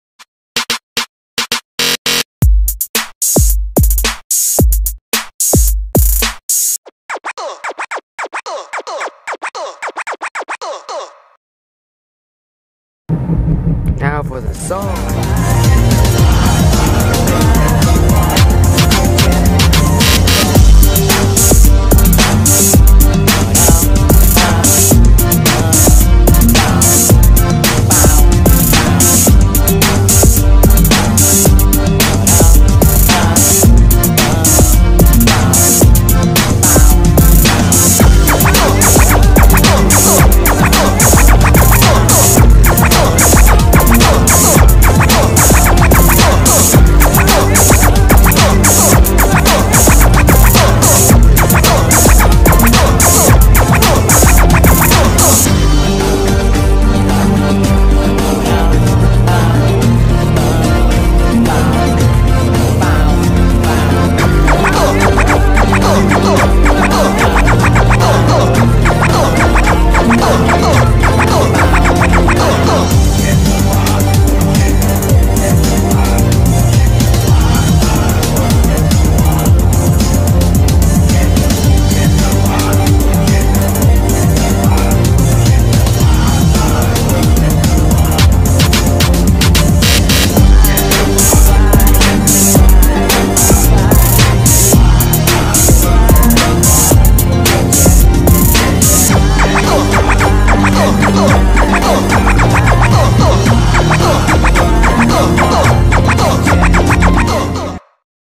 What If: Deedge and Turntisc were on Sebass 87's Mine Island?
Since Mine Island is getting animated soon, I figured I make a What-If video.

Turntisc belongs to Cosmoverse

Video by PowerDirector and KineMaster

Song edit by Beautiful Audio Editor